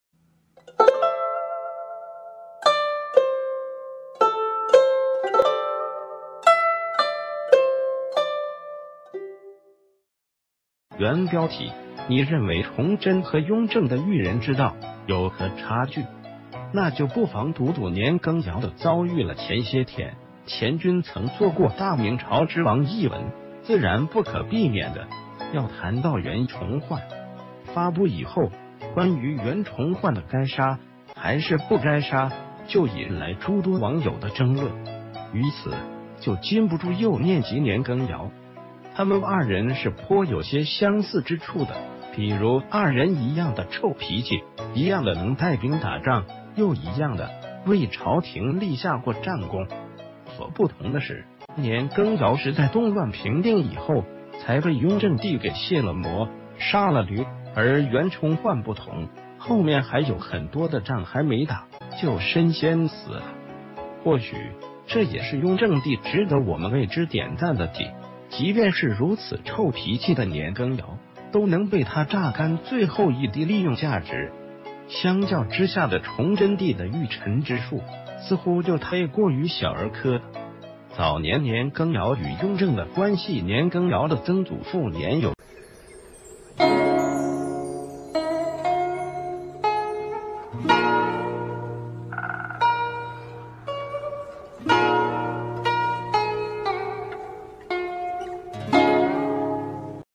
你認為崇禎和雍正的“馭人之道”有何差距？那就不妨讀讀年羹堯的遭遇了_四爺
原標題：你認為崇禎和...